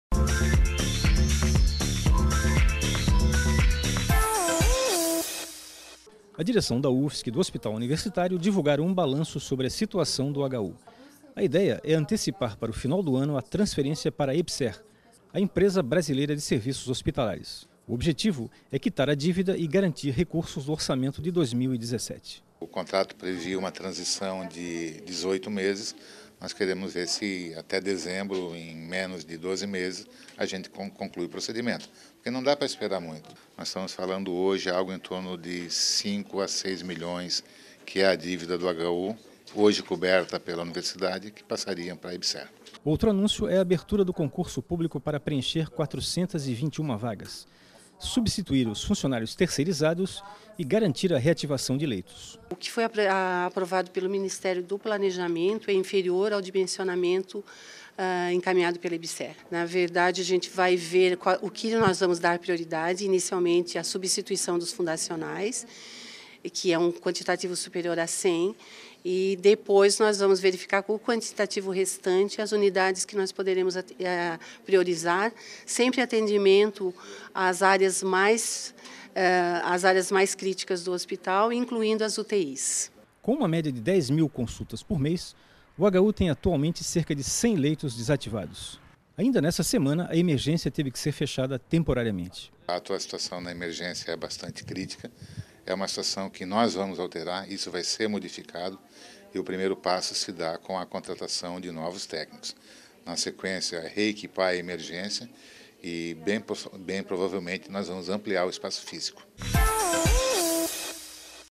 Universidade Já - UFSC e HU fazem balanço sobre transição para EBSERH e anunciam concurso público
A direção da UFSC e do Hospital Universitário divulgaram um balanço sobre a situação do HU. A ideia é antecipar para o final do ano a transferência para a EBSERH, a Empresa Brasileira de Serviços Hospitalares. O objetivo é quitar a dívida e garantir recursos do orçamento de 2017.

Outro anúncio é a abertura do concurso público para preencher 421 vagas, substituir os funcionários terceirizados e garantir a reativação de leitos.